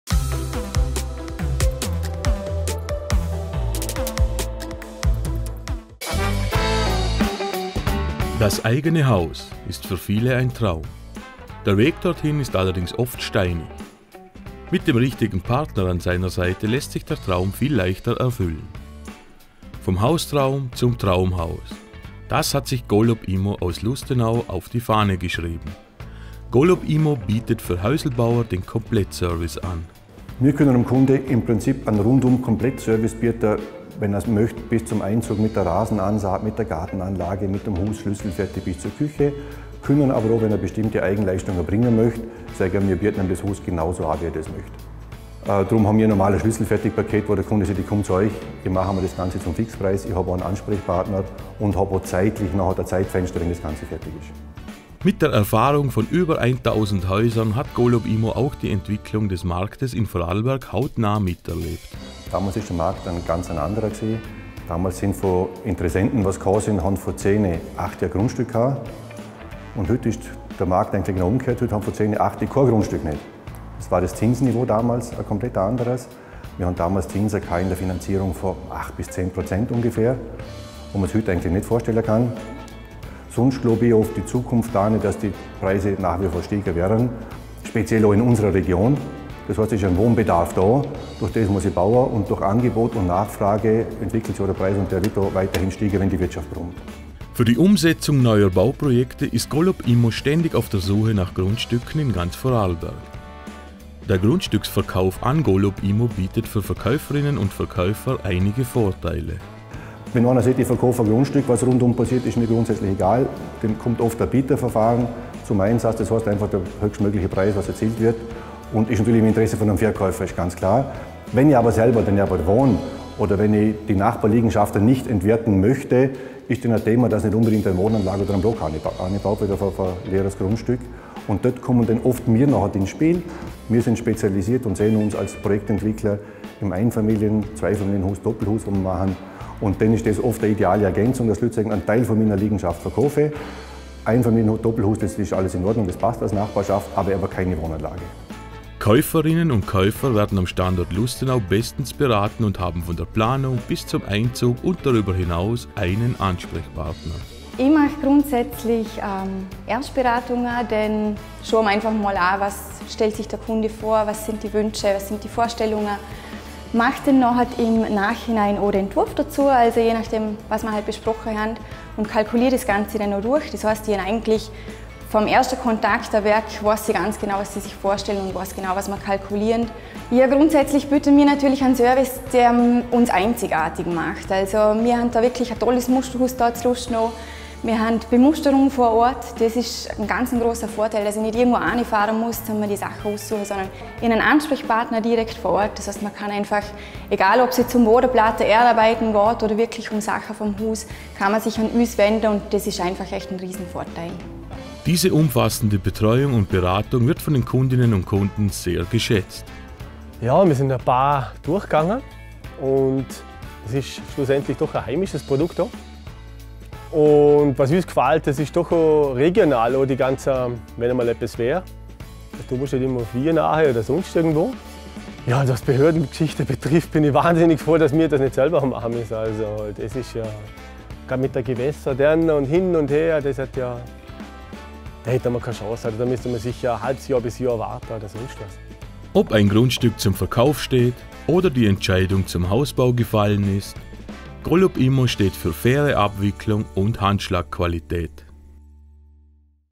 Unternehmen der Woche Golobimmo
"Schaffa, Schaffa, Hüsle baua" ist eine Tugend der Vorarlberger:innen. Einer, der Hausträume in Traumhäuser verwandelt ist Robert Golob. Mit seinem Unternehmen Golob Immo und der Erfahrung von über 1000 Häusern ist er der Spezialist für angehende Häuselbauer.